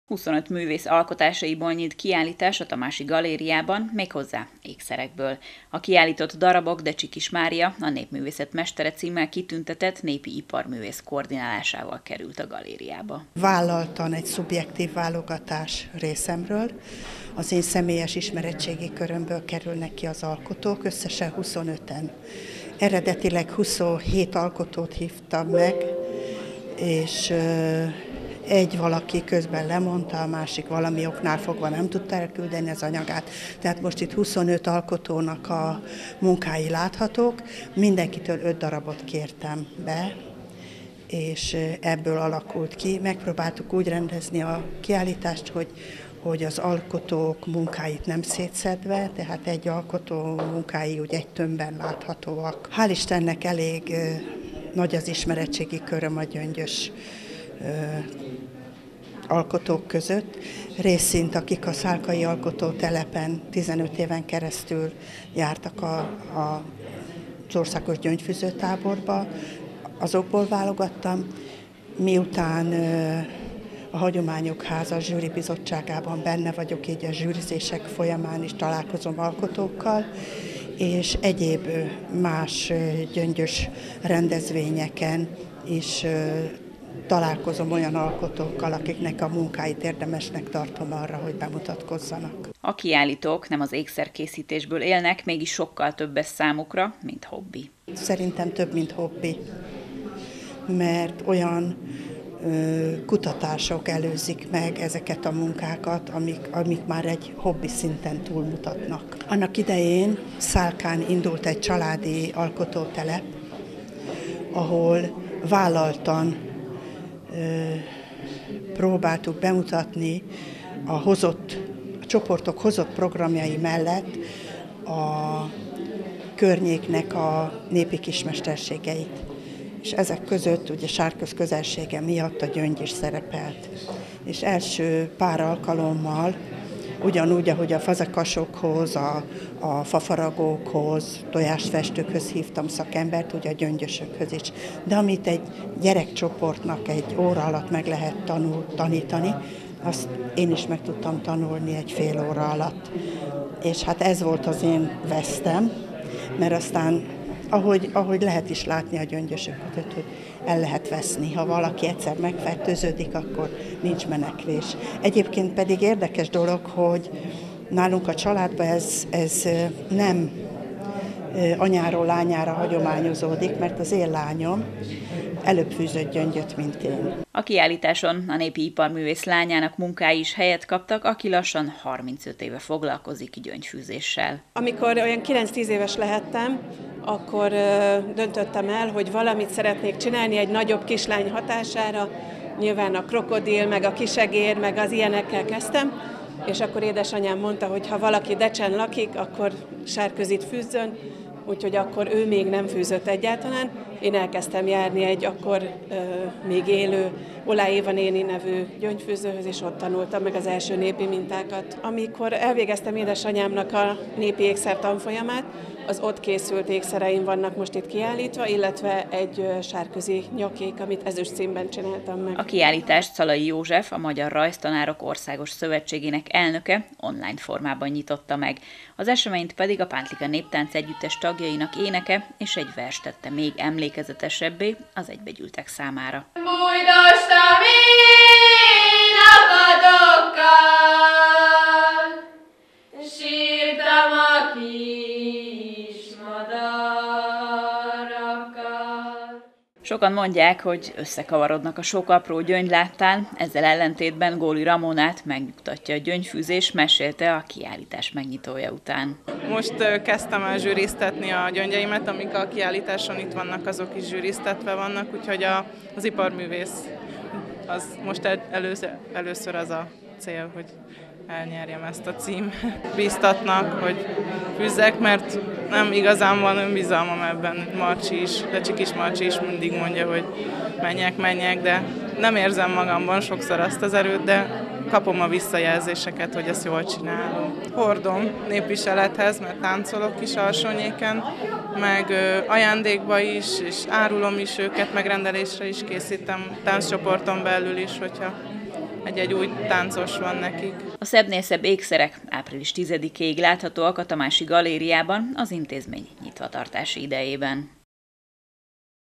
Gyöngyékszerek sokasága a Tamási Galériában
Gyerekként sokunk kezébe került damil, gyöngy és próbáltunk ezekből valamit alkotni, több-kevesebb sikerrel. Az biztos, hogy a Tamási Galériában látható kiállítás darabja a sikeres kategóriába tartoznak. Sok-sok kutatómunka, rengeteg tanulás és még több idő a gyöngyök társaságában, ami nélkülözhetetlen volt ahhoz, hogy megszülethessenek a kiállított darabok. Hölgyeim, a következő percek a biztos rácsodálkozást tartogatják.